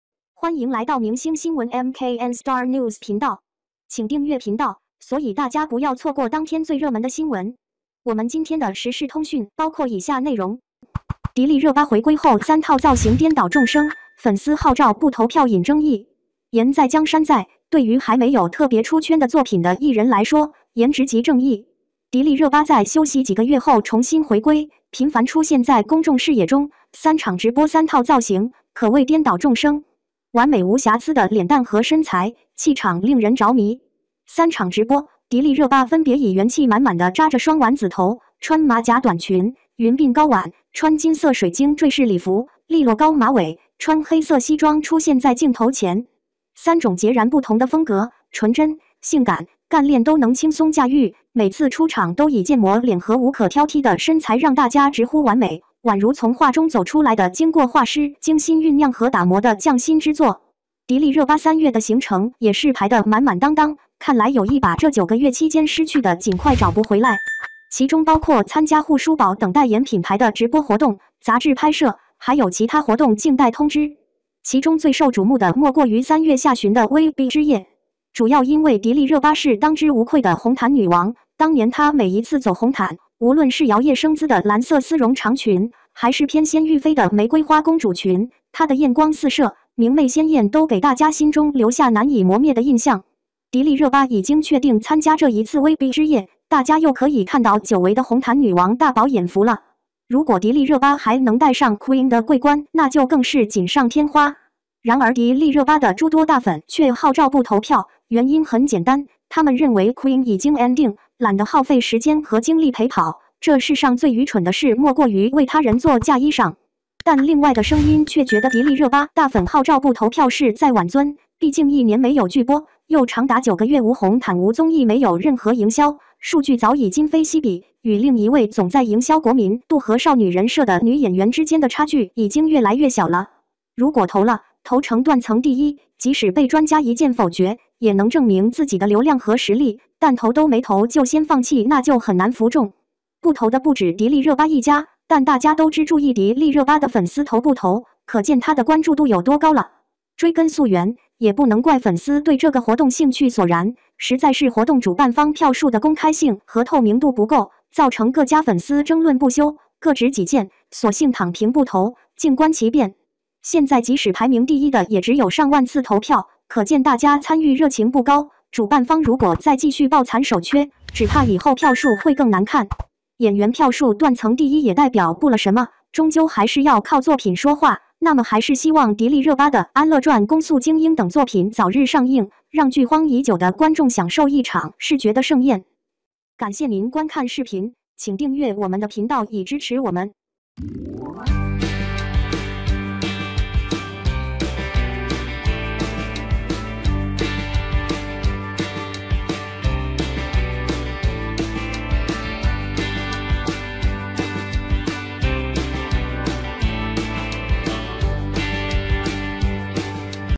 迪丽热巴回归后三套造型颠倒众生, 粉丝号召不投票引争议!
迪丽热巴回归后三套造型颠倒众生, 粉丝号召不投票引争议! | MKN Star News.